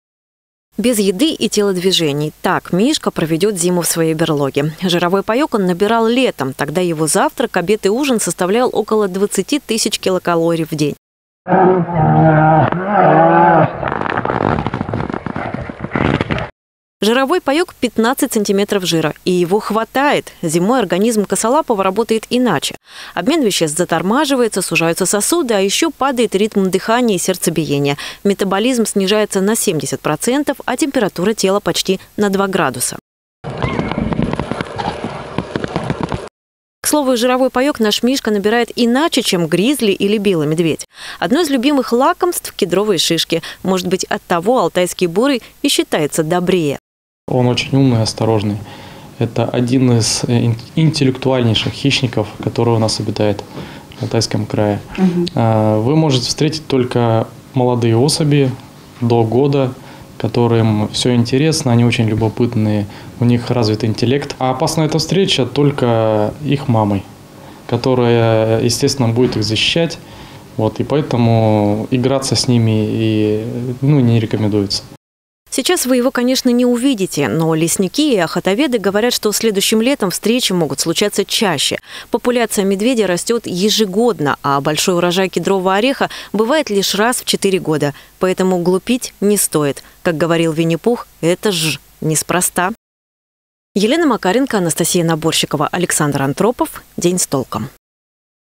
Сегодня – День медведя: в Алтайском крае проживает более тысячи косолапых
Сегодня – День медведя. И к нашему региону это событие имеет непосредственное отношение: в Алтайском крае проживает более тысячи косолапых. Но вот свой праздник хищник, который считается символом России, проводит в спячке. К слову, процесс этот очень необычный. Чтобы лучше понять нашего бурого медведя, расскажем о его особенностях в следующем материале